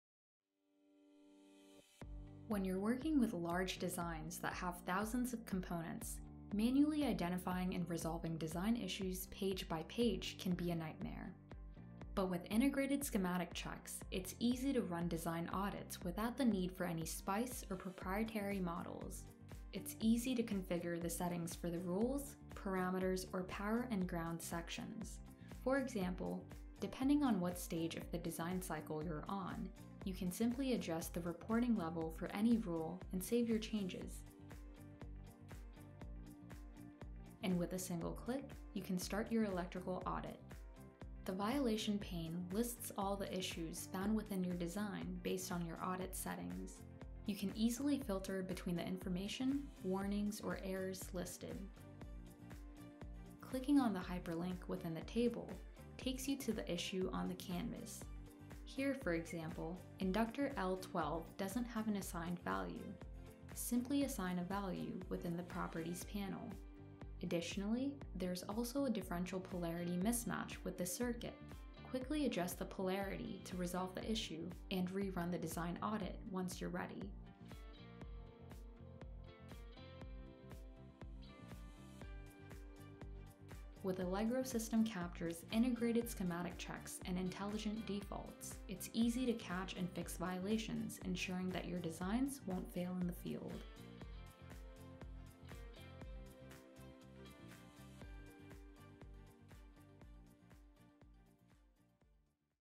Easily Run a Design Audit on Your Schematic | Allegro System Capture
With over 30 different schematic checks, easily analyze your design for reliability and identify and resolve any violations quickly.